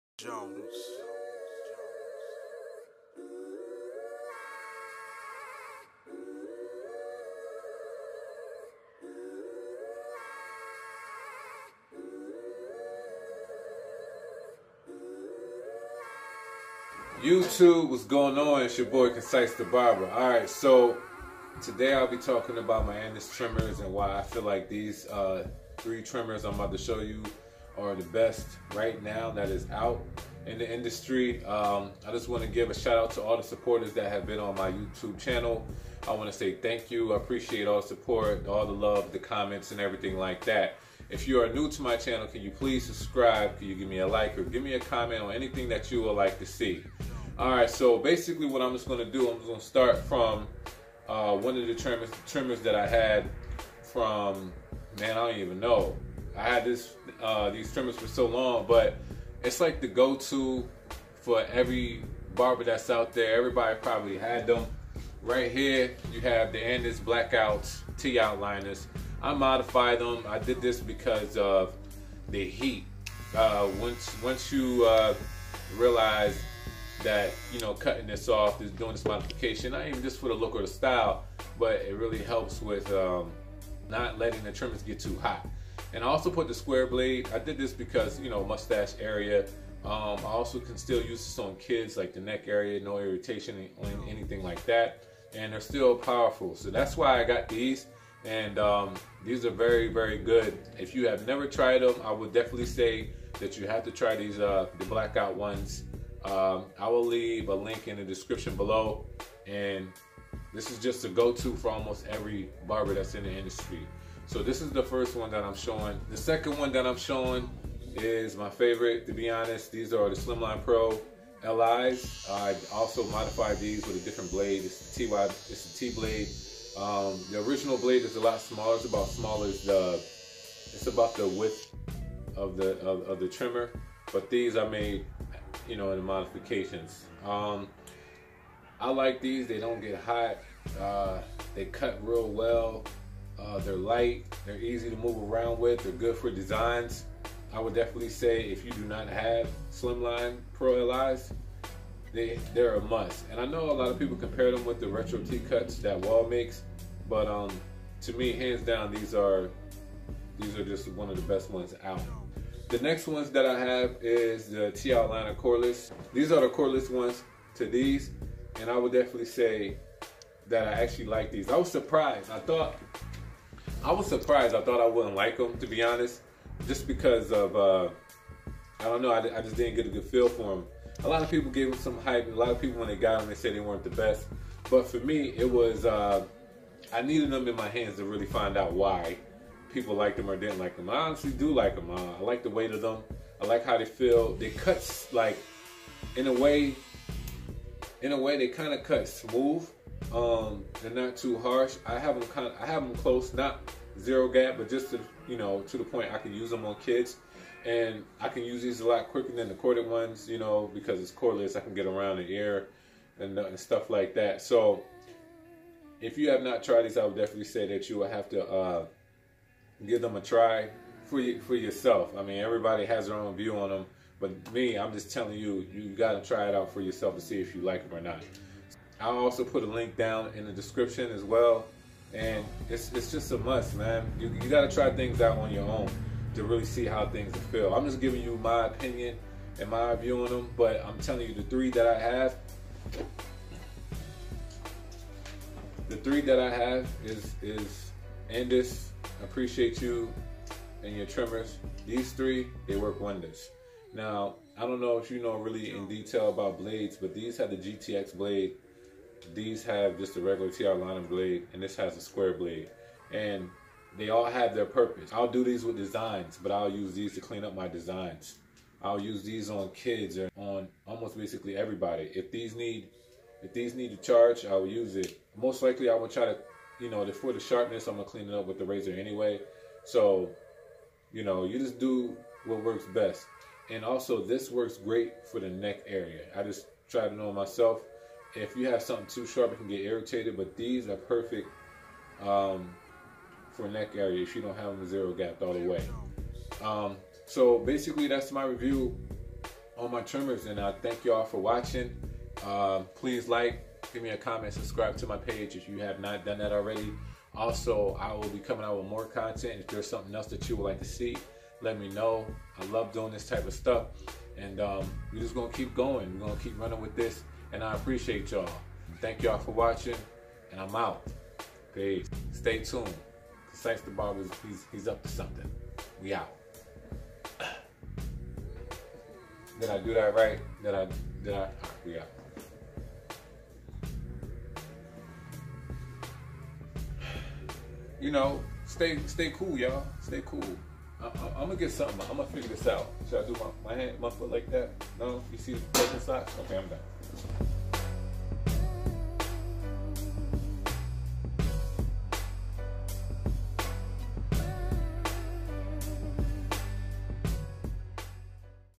BEST TRIMMERS BY ANDIS
The trimmers that I explain in my video are great for professional and at home use.

Andis T-Outliner Blackout

Andis T-Outliner GTX Blade

Andis Outliner II Blade

Andis SlimlinePro Li

Andis Cordless T-Outliner

Hudson Cape